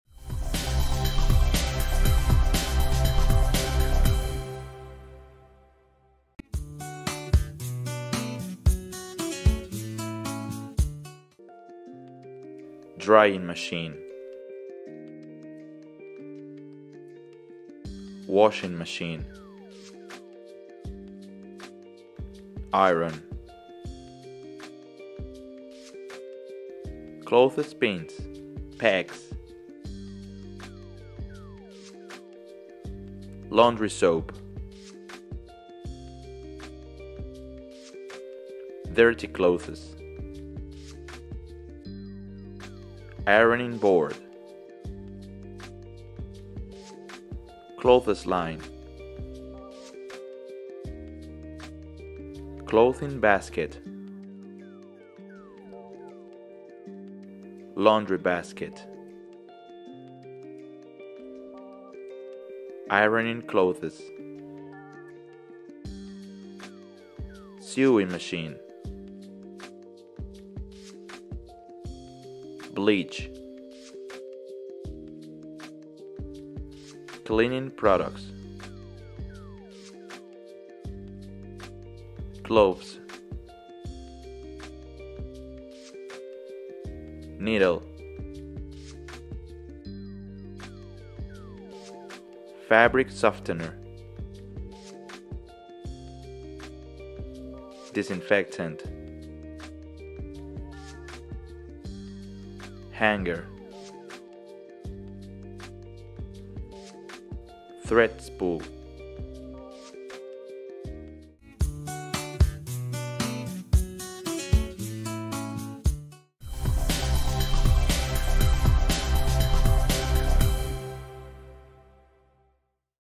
🧼 Objetos de la Lavandería en Inglés ✅ | CURSO DE INGLÉS GRATIS | Vocabualario de Inglés 🎽
Aprende Inglés Gratis con Nosotros en Academia Access ✅ En esta lección aprenderás el Vocabulario de la Lavandería en Inglés 🧼